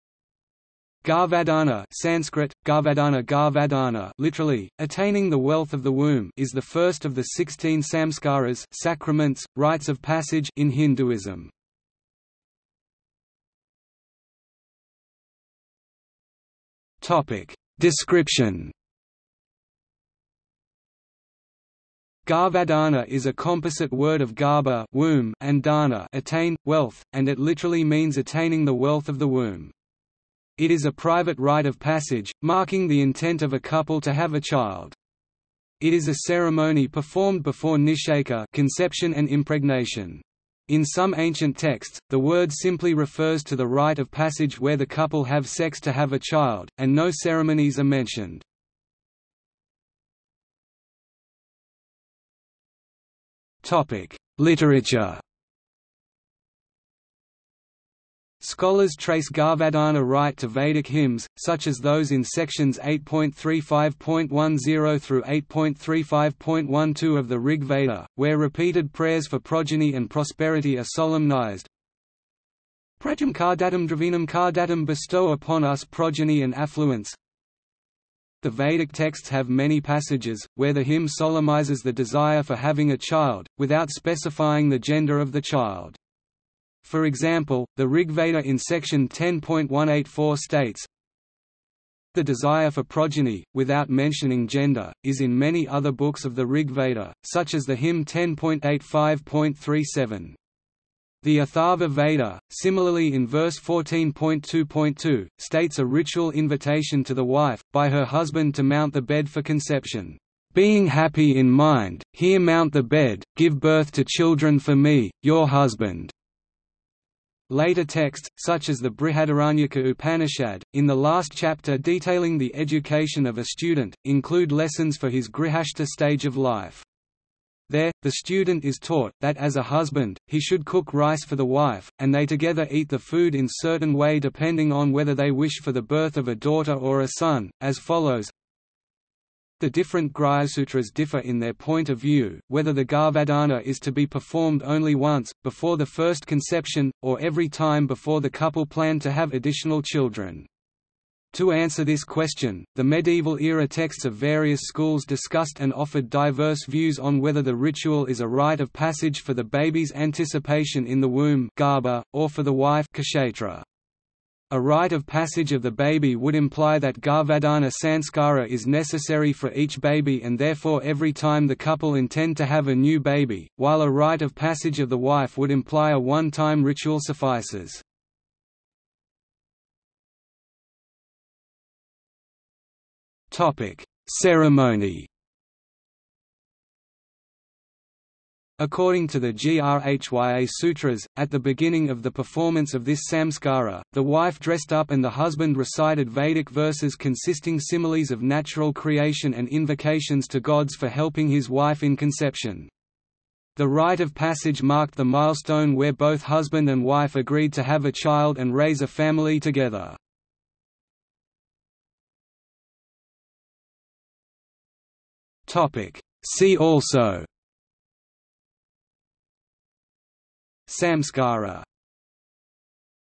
Garbhadhana | Wikipedia audio article
00:00:13 1 Description
00:00:50 1.1 Literature
00:02:59 2 Ceremony
00:03:31 3 See also

- Socrates

SUMMARY
Garbhadhana (Sanskrit: गर्भाधान, Garbhādhāna) (literally: attaining the wealth of the womb) is the first of the 16 saṃskāras (sacraments, rites of passage) in Hinduism.